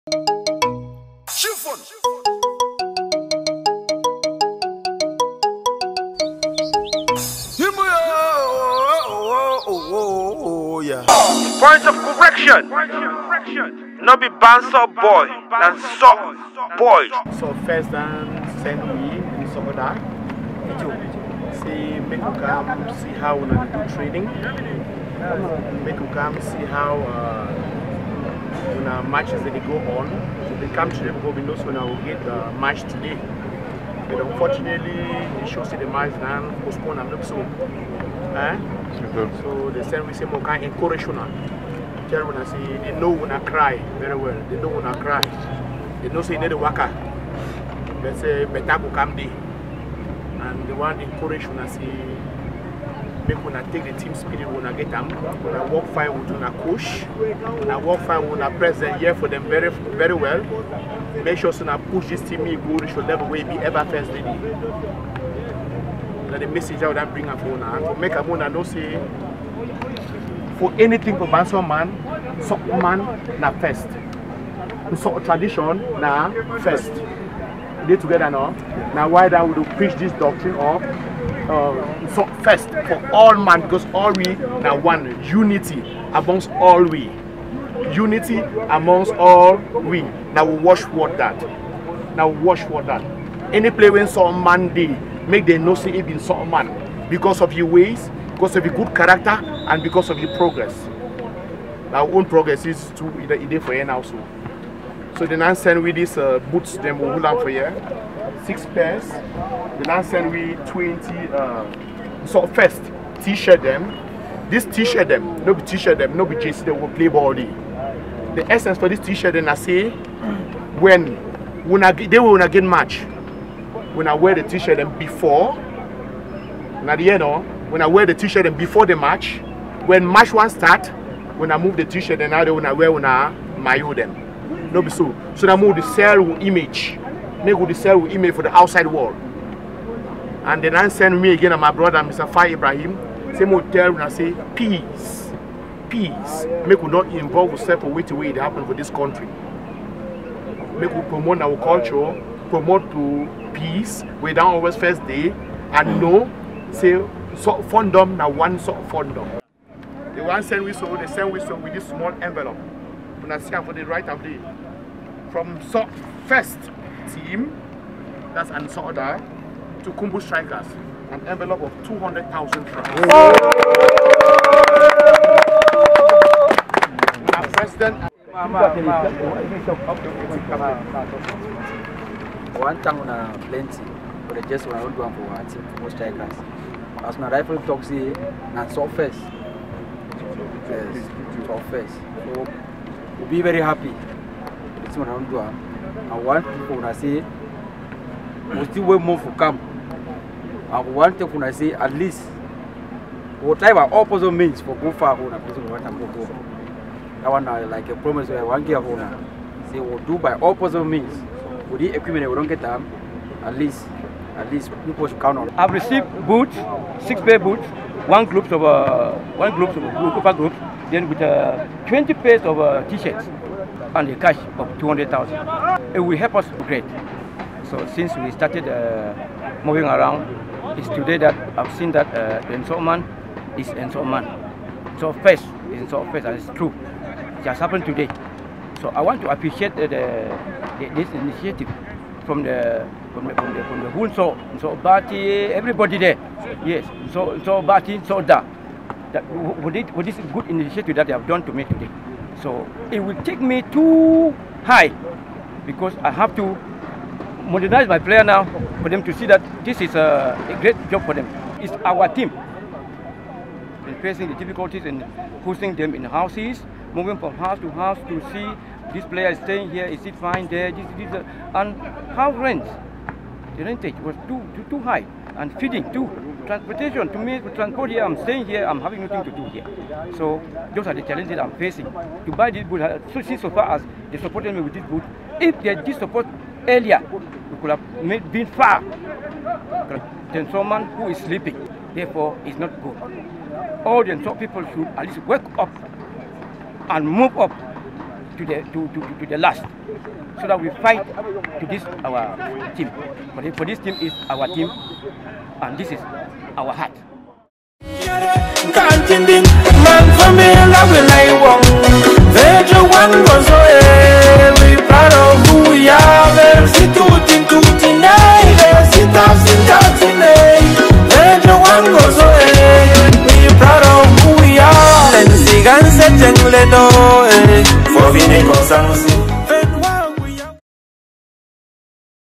(NSODA) in Partnership with NsoFirst assistance to Kumbo Strickers.
Nso Development Association (NSODA) in Partnership with NsoFirst donates sporting materials and the sum of 200,000frs to Kumbo Strickers on 06 June 2021 in Bamenda.

Fai Shulika theTeam's President/Coach and WIRBA Lius the Captain showed gratitude to the NsoFirst for their kind gesture and called on Well wishers of Kumbo Strickers as well as Nso Sons and daughters to do same in support of the team as they also promise their fans home and abroad a better performance in the days ahead.